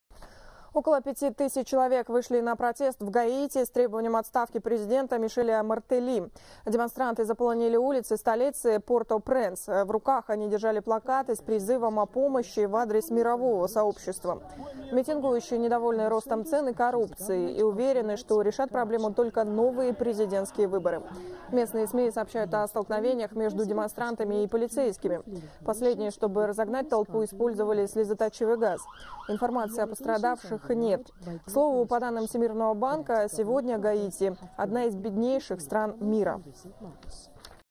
Массовые протесты с требованием отставки президента вспыхнули в Гаити
Около 5 тысяч человек вышли на протест в Гаити с требованием отставки президента Мишеля Мартелли.